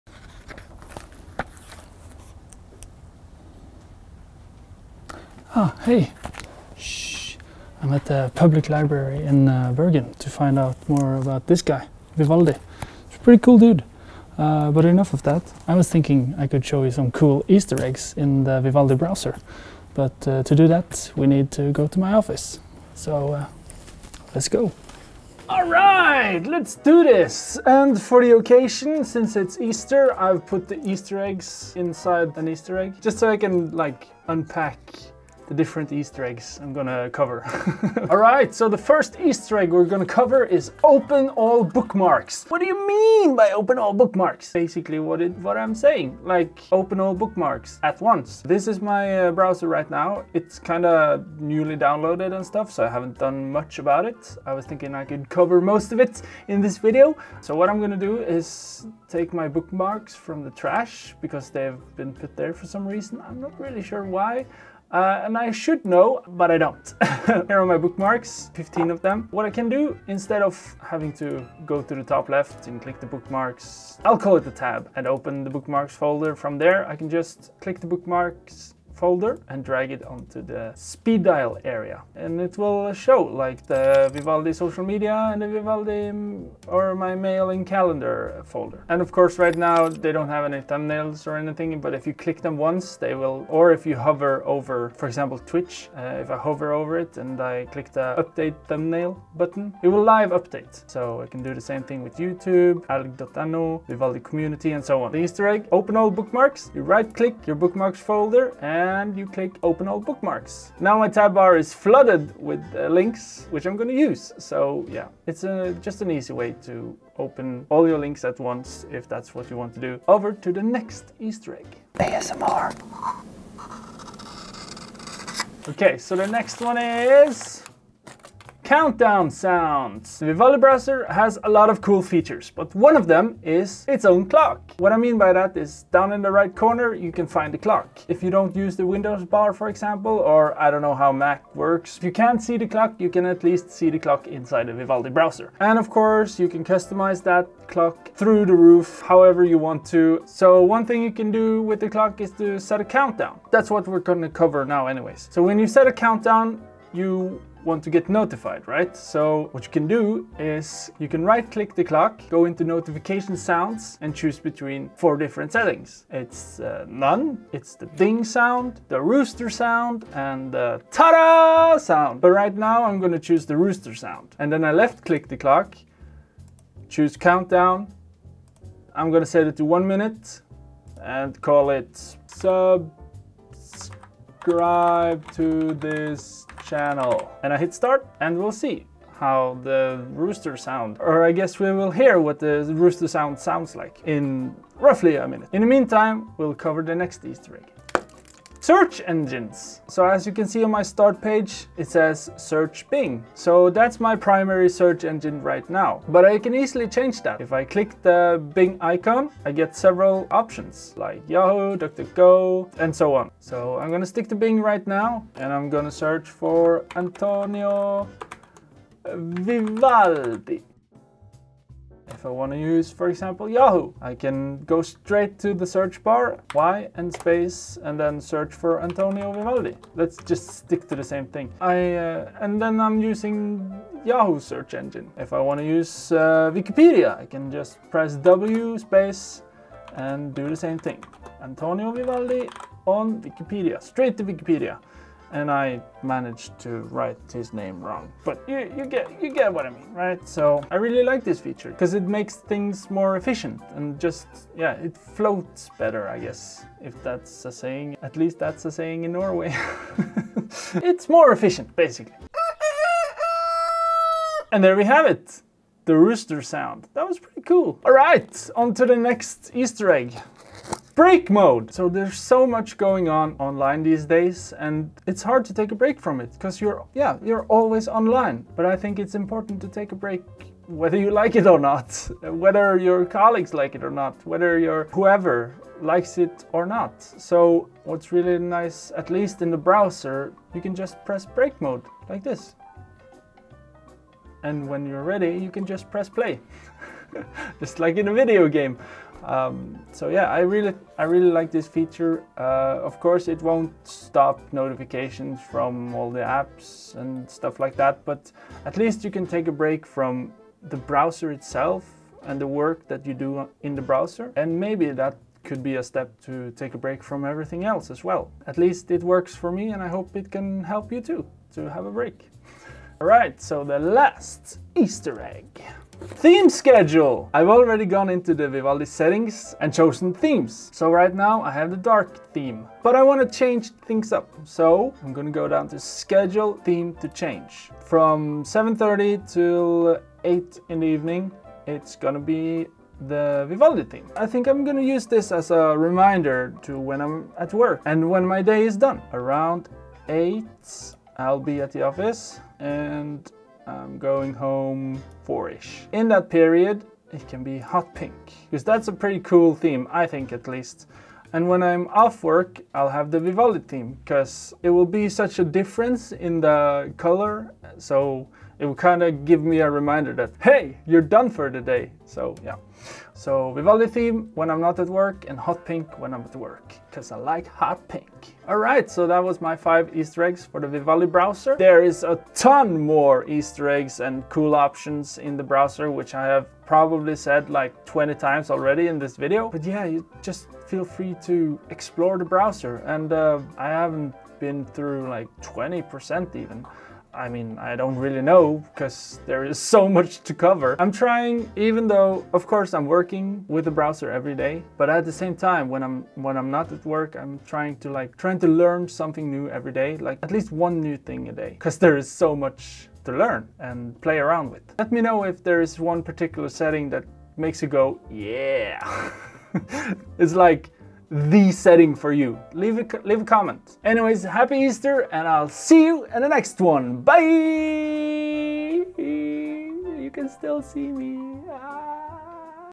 5 easter eggs in the Vivaldi Browser
Happy easter!  we are sending you on an Easter egg hunt with Prebz to discover 5 easter eggs in Vivaldi Browser.

0:41 Open ALL bookmarks : If you right-click one, you can open ALL the bookmarks inside at once.
2:08 Countdown sounds : Vivaldi has a clock if you right-click it and you go to the menu notification sound, you can change the sound.
3:19 Search Engines: You can set up a custom search engine on the Speed Dial and the Search Bar You just have to click on your favorite search engine to get many more options.
4:30 Break mode: Break Mode will pause playing video, click on the take a break button on the left side of the status bar.
5:26 Theme schedule: You can make Vivaldi themes change depending on the time. Themes section, enable the option of Schedule Theme to Change.